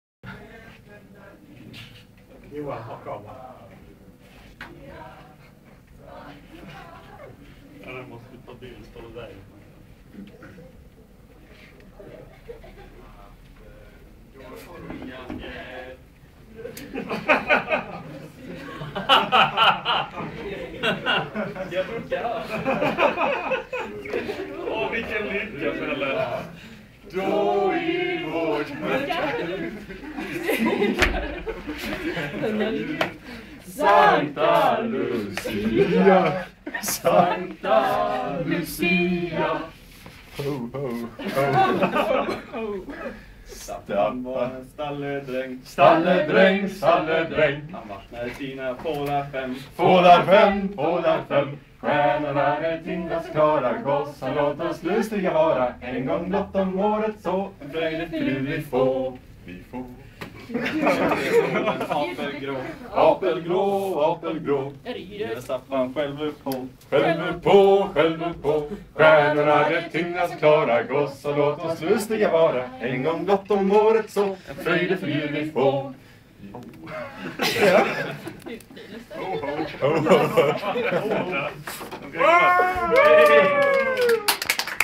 Prisjakts Luciafirande 2008
Pelle tog på sig rollen som Lucia.